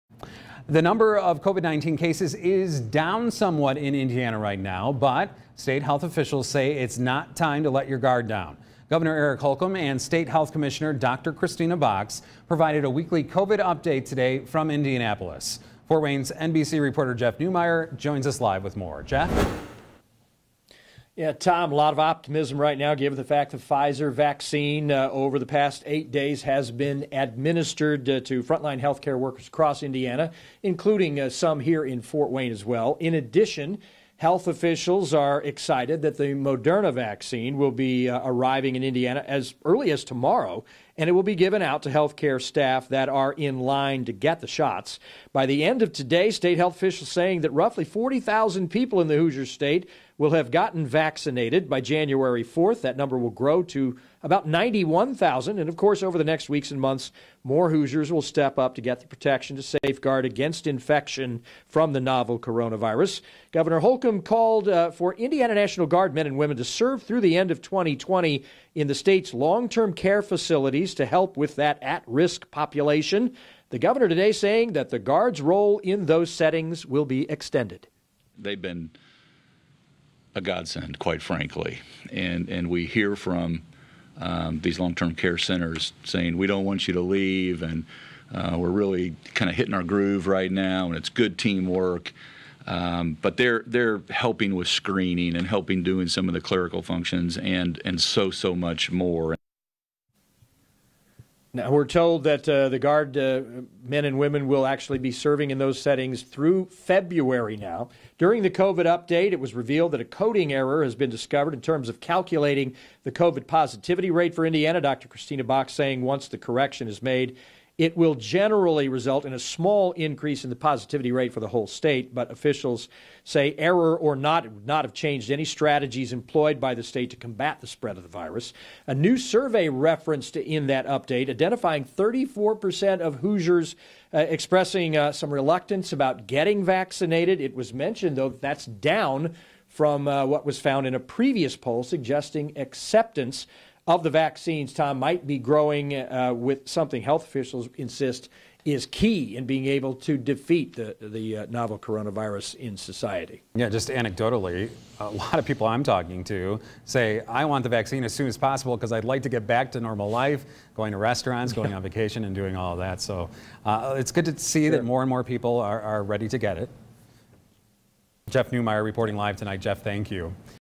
Gov, state health commissioner excited about COVID vaccines; press Hoosiers to keep their guard up
Indiana Governor Eric Holcomb is encouraged about a growing availability of COVID vaccines in the Hoosier state.